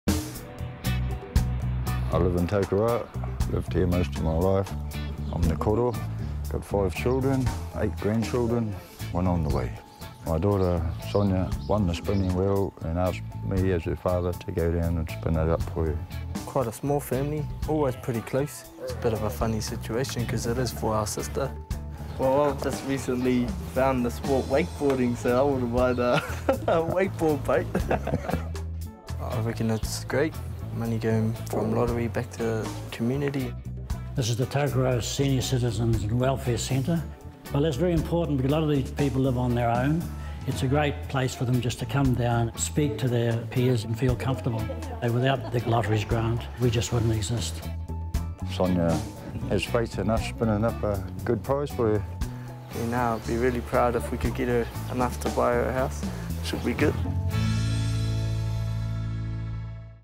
Lotto's Winning Wheel draw 1337 19/01/2013 Leon & Steve from Tokoroa.
Meet this weekend's nominated Winning Wheel spinners, father and son, Leon and Steve from Tokoroa.

They are spinning on behalf of their sister/daughter, new mother Sonya-Lee, who offered them the trip to Wellington to spin the wheel at the live Lotto draw.

We also get to meet the wonderful people that are part of the Tokoroa Senior Citizens and Welfare Centre. A regular Lottery grant recipient, the centre provides services that include meals, bingo, bowling, and crafts -- and there are a few card sharks that hang out there too!